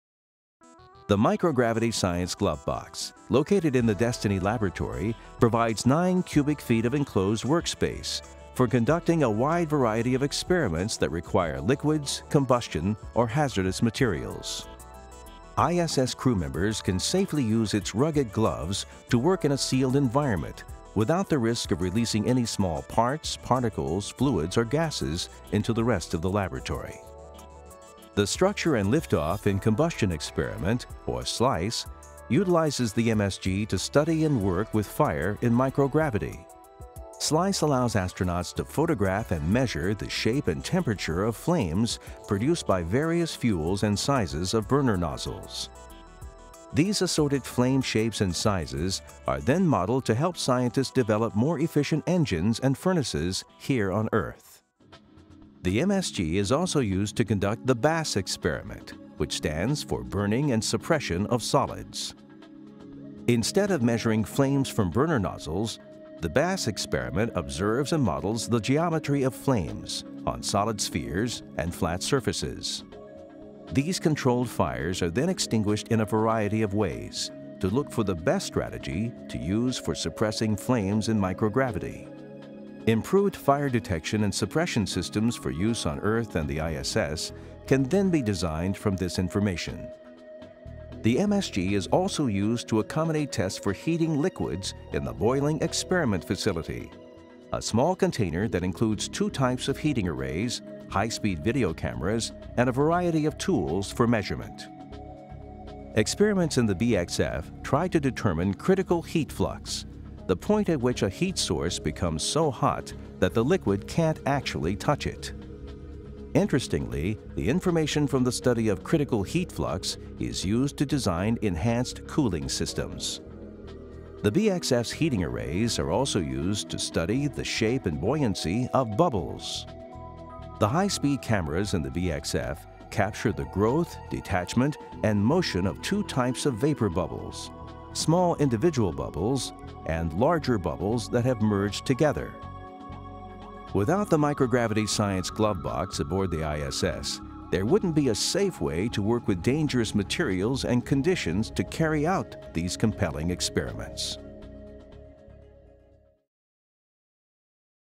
Microgravity Science Glovebox
This video is one of a series of short features about some of the experiment racks on station. This video focuses on the Microgravity Science Glovebox (MSG) and features the Structure and Liftoff in Combustion Experiment (SLICE) and the Burning and Suppression of Solids (BASS).